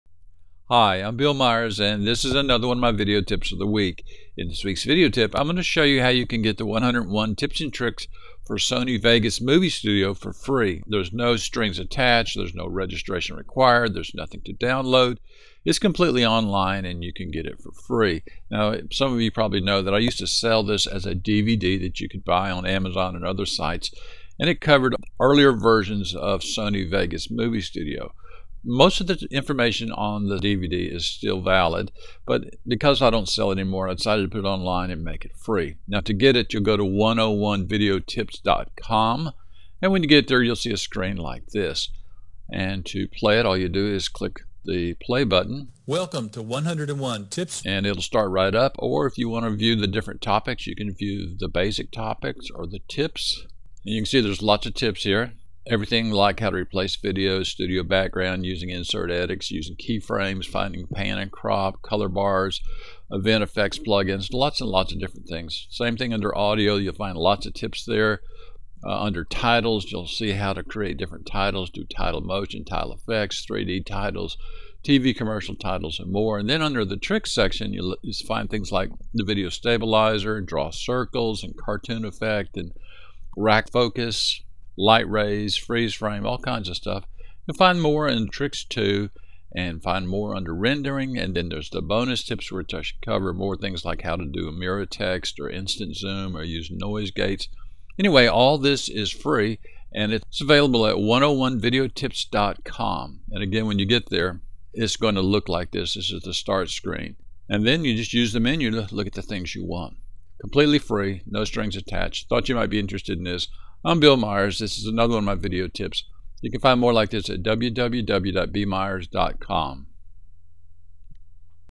Over 100 free tips and tricks for Vegas Movie Studio
I've uploaded the full 101 Tips, Tricks and Techniques for Vegas Movie Studio DVD and you can view it for free. No strings attached, no registration required, no ads.  Totally free.  Over 125 video tips that work with almost every version of Vegas Movie Studio and Vegas Pro.